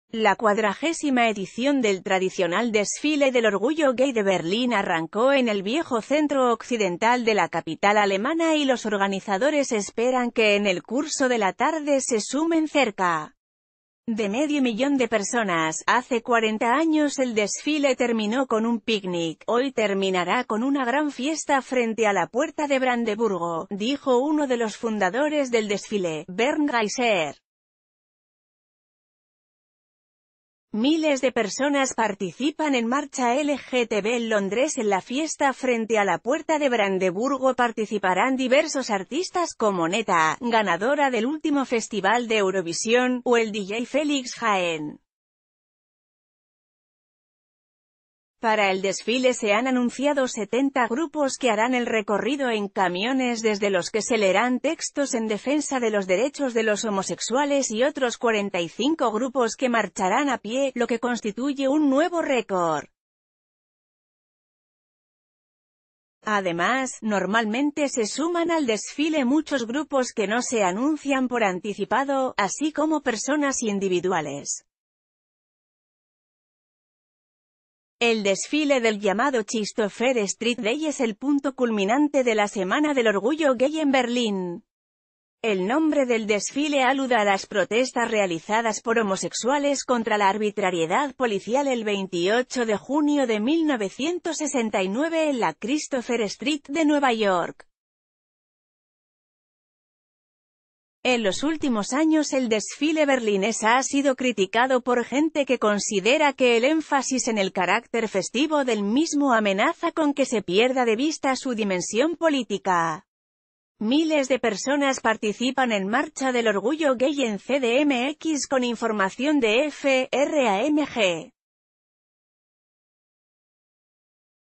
Berlín celebra multitudinario desfile del orgullo gay
La cuadragésima edición del tradicional desfile del orgullo gay de Berlín arrancó en el viejo centro occidental de la capital alemana y los organizadores esperan que en el curso de la tarde se sumen cerca de medio millón de personas.“Hace 40 años el desfile terminó con un pícnic, hoy terminará con u...